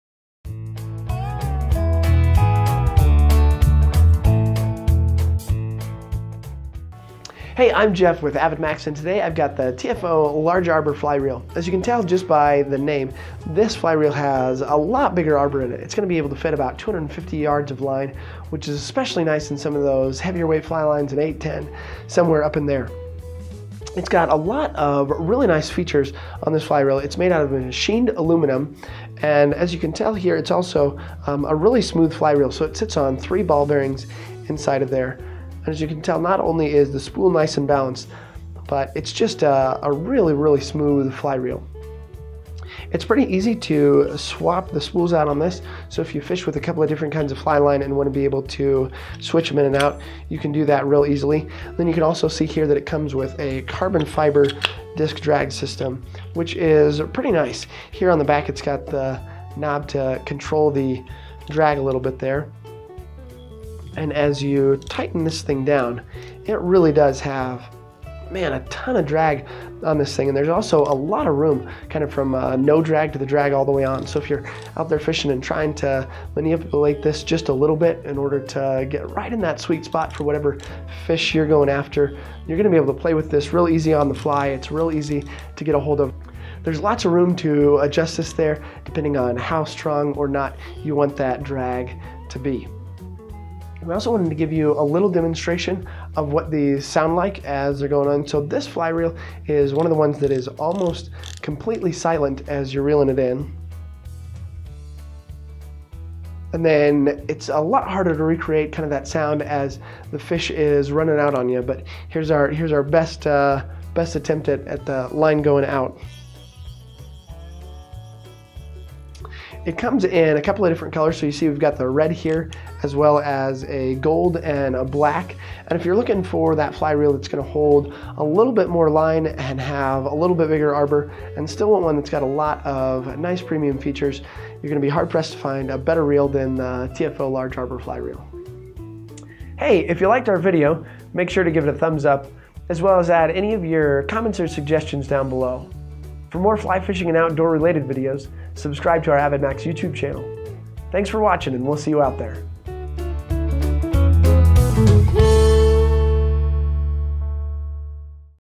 TFO Large Arbor Reels Review | AvidMax
In this AvidMax product review, watch as Jeff highlights the key features of the TFO Large Arbor Reels!